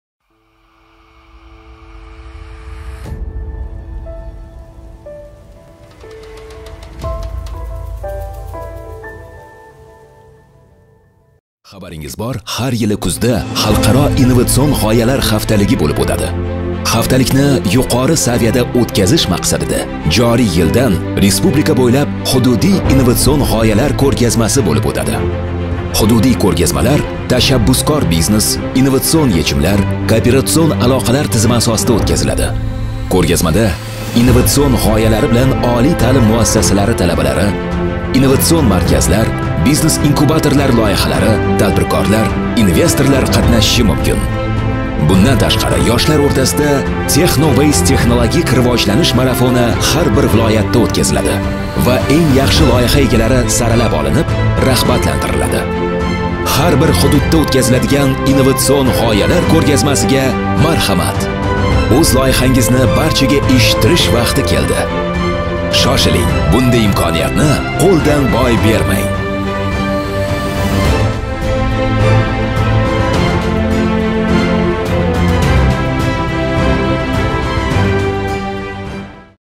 “InnoWeek.uz” - Xalqaro innovatsion g‘oyalar haftaligining hududiy bosqichiga start berildi
“InnoWeek.uz” - Xalqaro innovatsion g‘oyalar haftaligining hududiy bosqichiga start berildi. Bugundan boshlab respublika bo‘ylab bosqichma-bosqich hududiy innovatsion g‘oyalar ko‘rgazmasi bo‘lib o‘tadi. Bugun navbat Andijonga!